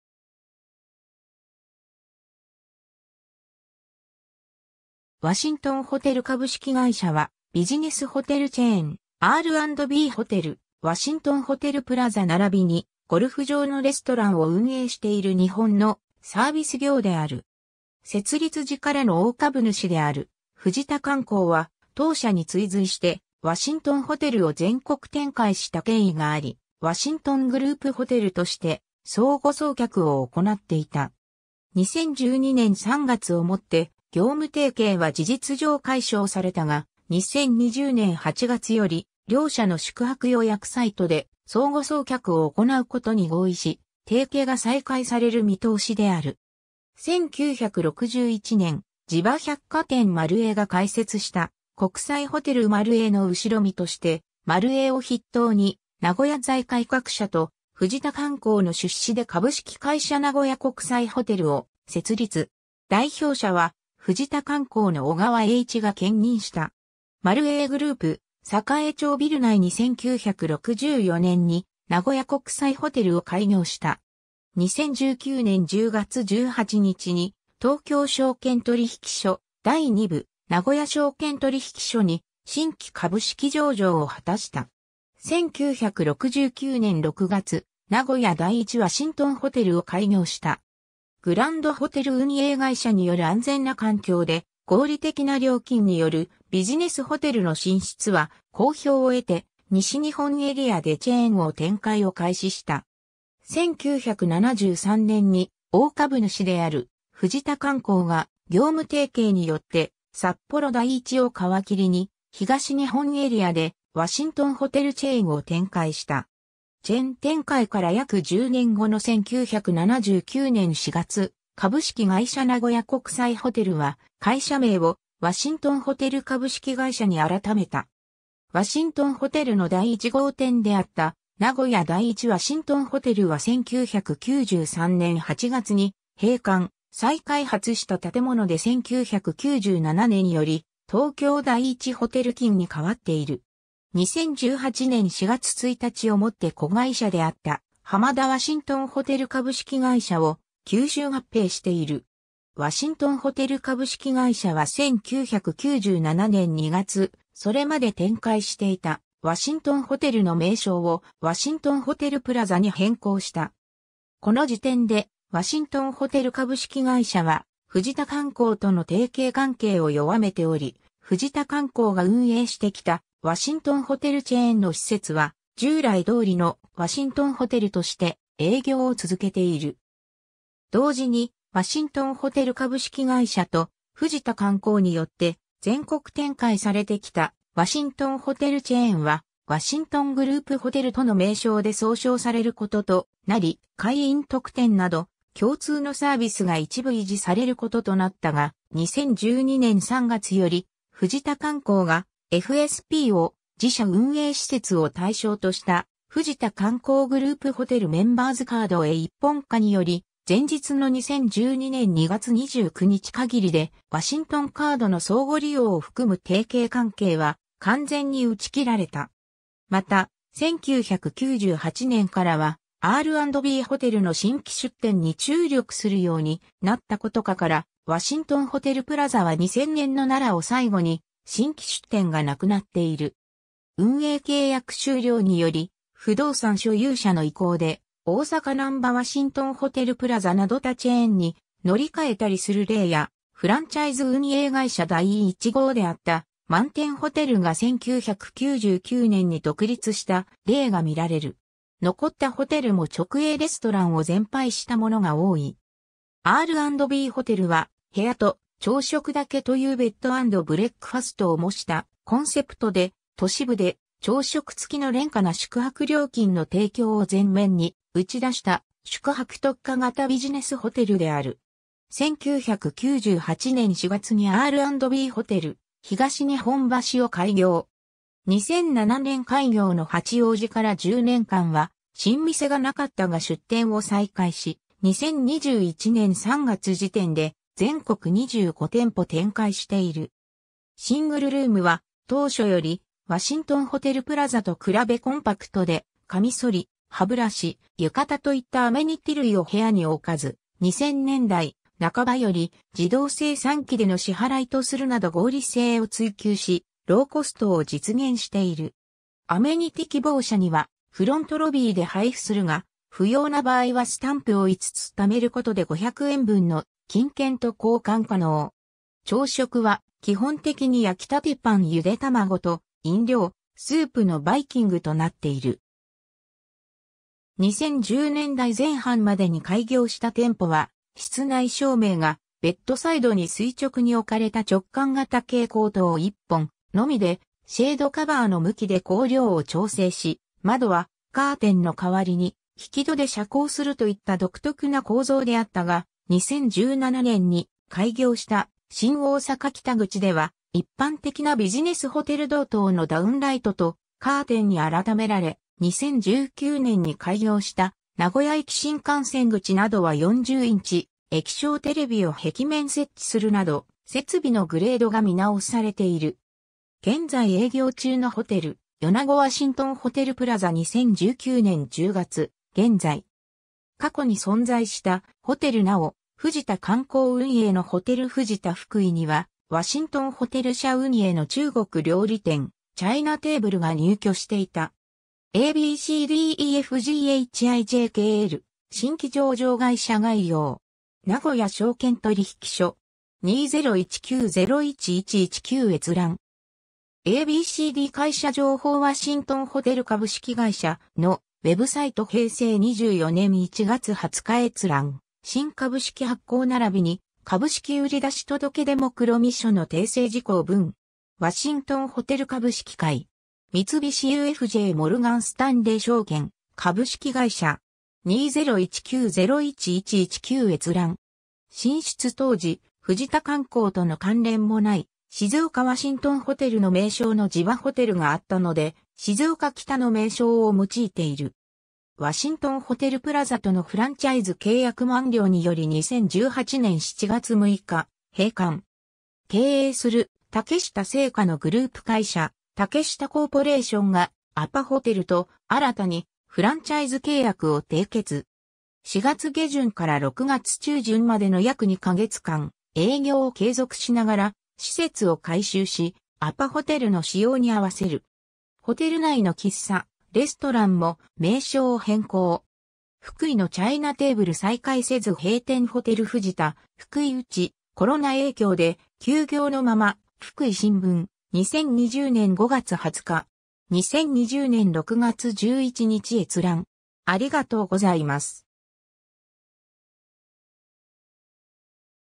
ワシントンホテル (愛知県の企業)
錦_(名古屋市)
ワシントンホテル株式会社（英称:Washington Hotel Corporation）は、ビジネスホテルチェーン「R&Bホテル」「ワシントンホテルプラザ」ならびにゴルフ場のレストランを運営している日本のサービス業である。
 設立時からの大株主である藤田観光は、当社に追随してワシントンホテル (藤田観光)を全国展開した経緯があり、「ワシントングループホテル」として相互送客を行っていた。
2012年3月をもって業務提携は事実上解消されたが、2020年8月より、両社の宿泊予約サイトで相互送客を行うことに合意し、提携が再開される見通しである。
 1961年（昭和36年）、地場百貨店丸栄が開設した「国際ホテル丸栄」の後身として、丸栄を筆頭に名古屋財界各社と藤田観光の出資で株式会社名古屋国際ホテルを設立。
代表者は藤田観光の小川栄一 (実業家)が兼任した。
丸栄グループ栄町ビル内に1964年（昭和39年）に「名古屋国際ホテル」を開業した。
 2019年10月18日に東京証券取引所第2部・名古屋証券取引所に新規株式上場を果たした。
 1969年（昭和44年）6月、「名古屋第1ワシントンホテル」（当初の名称は「国際ホテル別館ワシントンホテル」）を開業した。
グランドホテル運営会社による安全な環境で合理的な料金によるビジネスホテルの進出は好評を得て、西日本エリアでチェーンを展開を開始した。
1973年に大株主である藤田観光が業務提携によって札幌第1を皮切りに、東日本エリアでワシントンホテルチェーンを展開した。
チェーン展開から約10年後の1979年（昭和54年）4月、株式会社名古屋国際ホテルは、会社名をワシントンホテル株式会社に改めた。
 ワシントンホテルの第一号店であった名古屋第1ワシントンホテルは1993年（平成5年）8月に閉館、再開発した建物で1997年より「東京第一ホテル錦」に変わっている。
 2018年（平成30年）4月1日をもって子会社であった浜田ワシントンホテル株式会社を吸収合併している。
 ワシントンホテル株式会社は1997年（平成9年）2月、それまで展開していたワシントンホテルの名称を「ワシントンホテルプラザ」に変更した。
この時点でワシントンホテル株式会社は藤田観光との提携関係を弱めており、藤田観光が運営してきたワシントンホテルチェーンの施設は、従来通りの「ワシントンホテル」として営業を続けている。
同時に、ワシントンホテル株式会社と藤田観光によって全国展開されてきたワシントンホテルチェーンは、「ワシントングループホテル」との名称で総称されることとなり、会員特典など共通のサービスが一部維持されることとなったが、2012年3月より藤田観光がFSPを自社運営施設（のちのWHGホテルグループ）を対象とした「藤田観光グループホテル・メンバーズカード」へ一本化により、前日の2012年2月29日限りでワシントン・カードの相互利用を含む提携関係は完全に打ち切られた。
 また、1998年からはR&Bホテルの新規出店に注力するようになったことかから、ワシントンホテルプラザは2000年の奈良を最後に新規出店がなくなっている。
運営契約終了により、不動産所有者の意向で「大阪なんばワシントンホテルプラザ」など他チェーンに乗り換えたりする例や、フランチャイズ運営会社第一号であったマンテンホテルが1999年に独立した例が見られる。
残ったホテルも直営レストランを全廃した（朝食のみ提供）ものが多い。
 R&Bホテルは、「部屋と朝食だけ」というベッド・アンド・ブレックファストを模したコンセプトで、都市部で朝食付きの廉価な宿泊料金の提供を前面に打ち出した宿泊特化型ビジネスホテルである。
1998年（平成10年）4月にR&Bホテル東日本橋を開業。
2007年開業の八王子から10年間は新店がなかったが出店を再開し、2021年3月時点で全国25店舗展開している。
 シングルルームは当初よりワシントンホテルプラザ（12㎡前後）と比べコンパクト（9㎡）で、カミソリ・歯ブラシ・浴衣といったアメニティ類を部屋に置かず、2000年代半ばより自動精算機での支払いとするなど合理性を追求し、ローコストを実現している。
アメニティ希望者にはフロント・ロビーで配布するが、不要な場合はスタンプを5つ貯め...